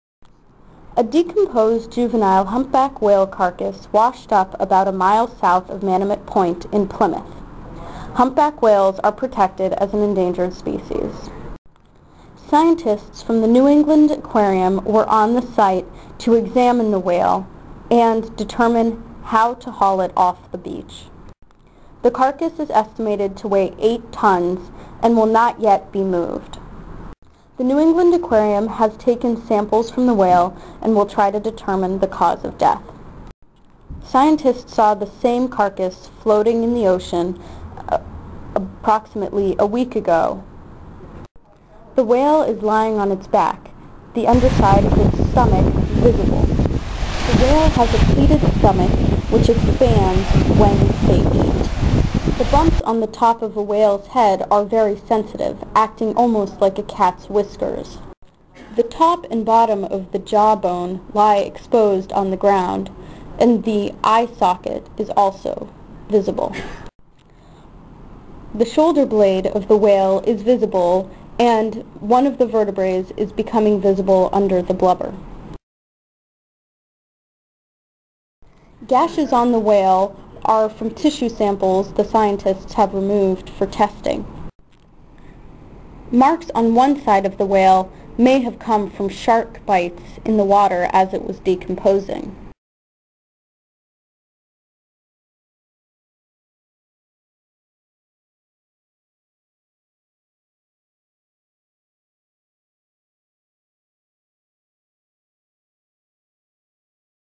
Humpback whale carcass washes up in Plymouth
A decomposed juvenile humpback whale carcass washed up about a mile south of Manomet Point in Plymouth.